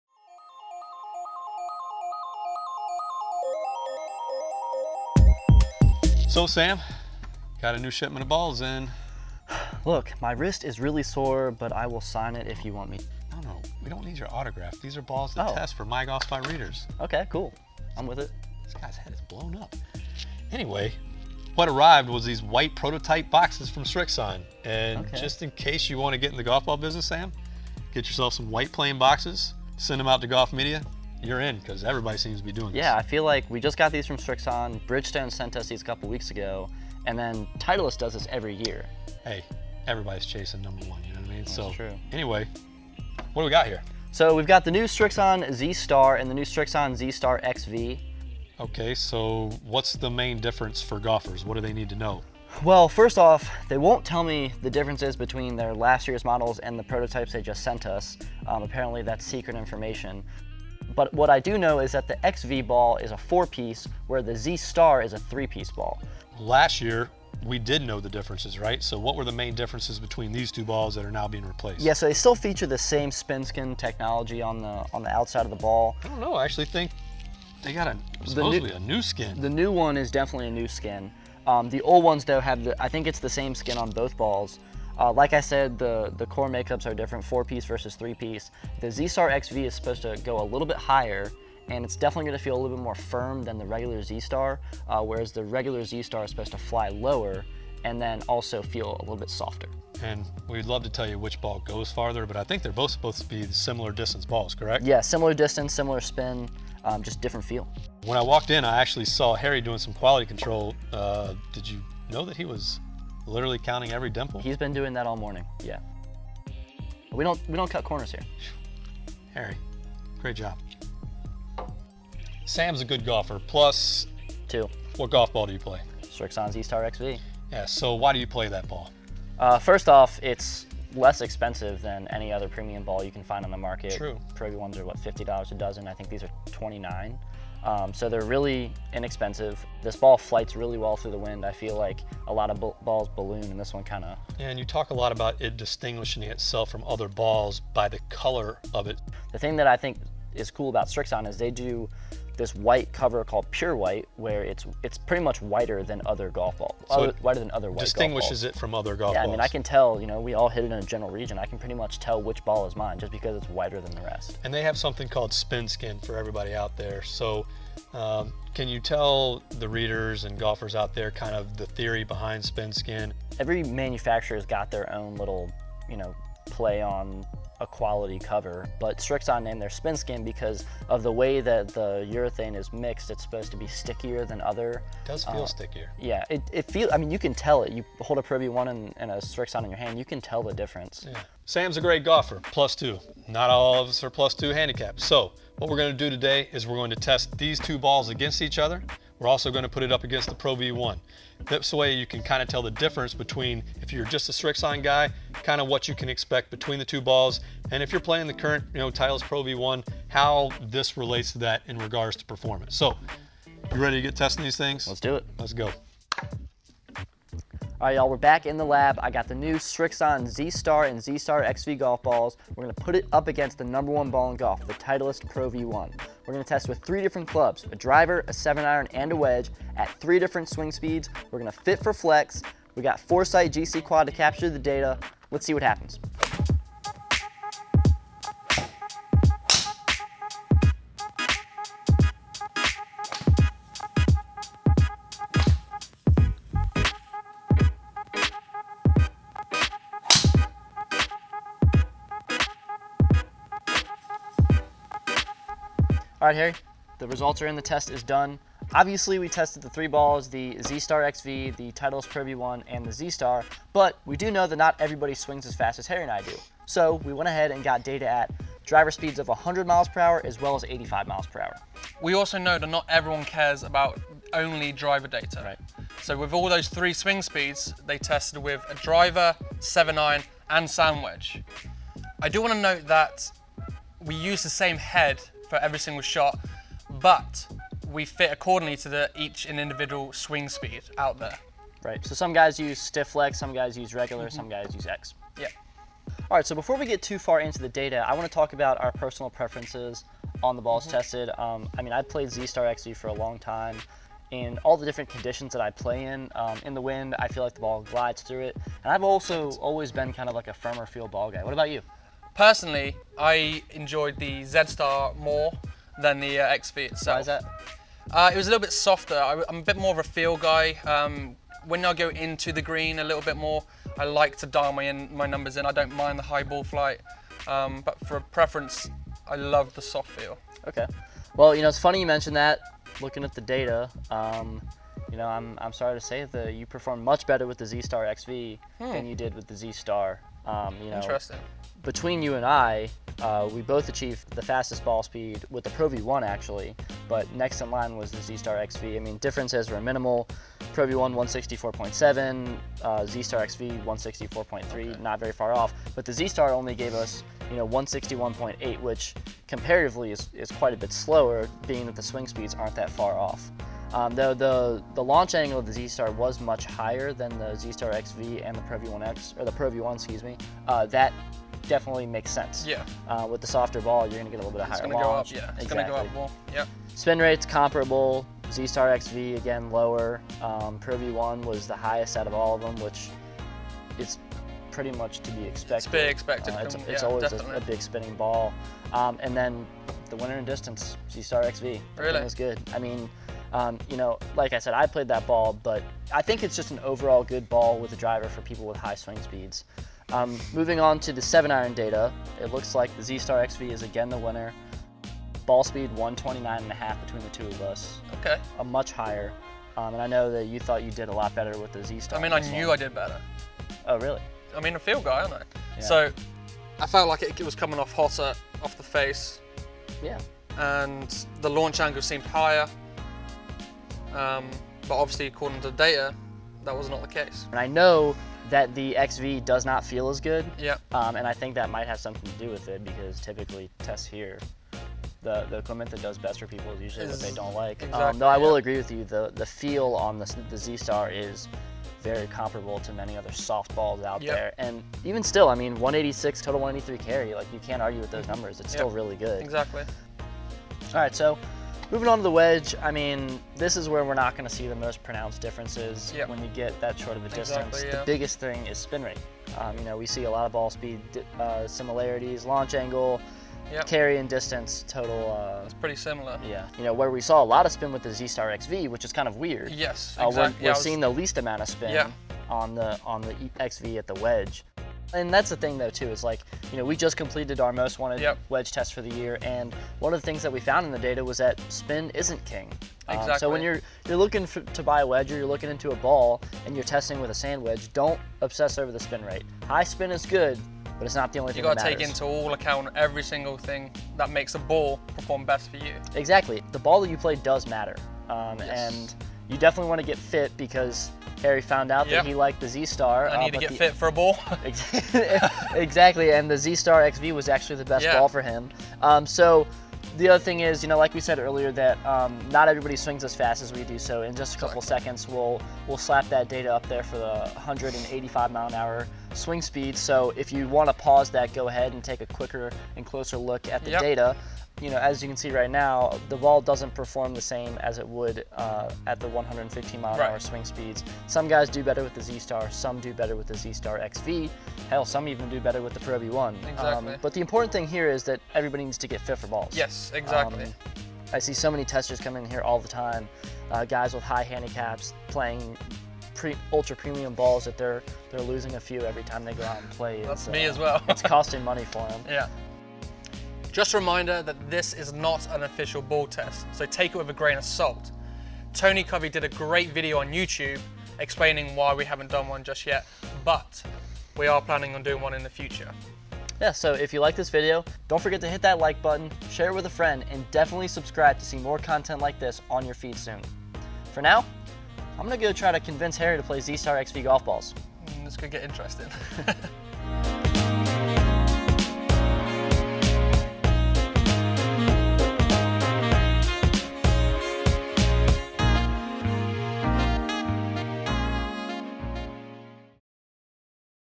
TEST DRIVE: 2019 SRIXON Z STAR & Z STAR XV GOLF BALL REVIEW
2019 SRIXON Z STAR & Z STAR XV GOLF BALL REVIEW

Editor's Note: The chart at 8:57 (7-Iron @ 85mph) shows incorrect information, the proper chart can be seen

Someone at Srixon sent MyGolfSpy a few sleeves of their prototype Z Stars for next year. The only logical conclusion? Put them to the test, of course!

Today we look at the numbers from the new Z Star and Z Star XV while also comparing their stats to the renowned Titleist Pro-V1.